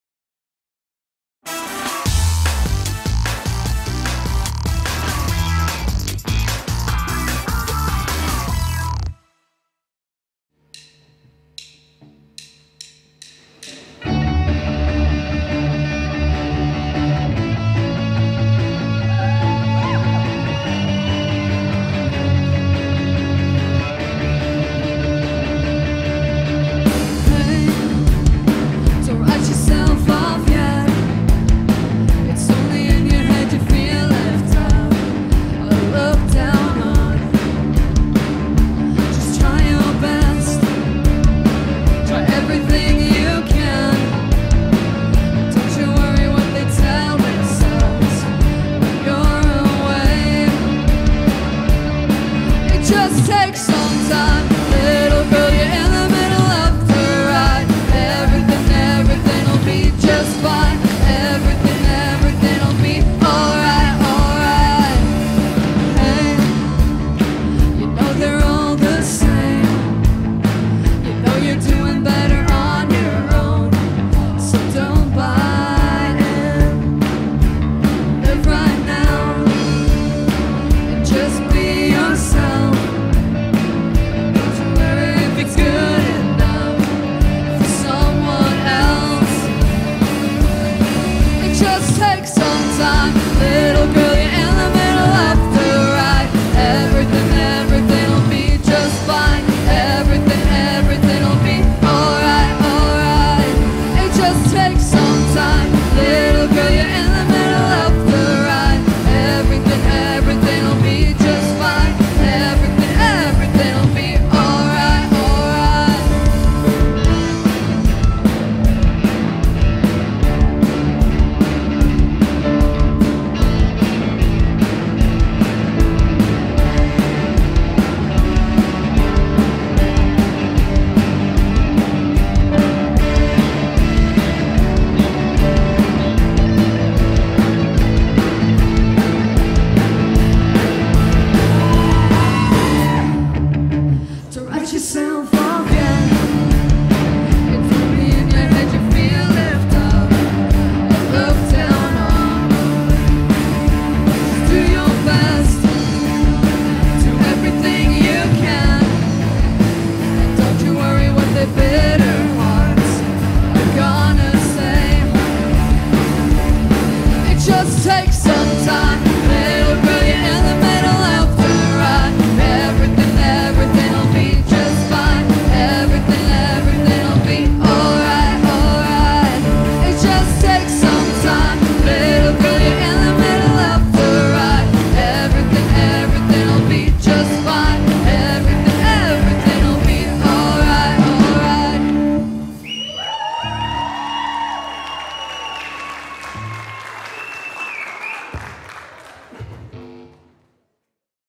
Jizelle Jones & Louis Payne with The Middle!
This is a "Live" recording of our The Gig concert series 2023.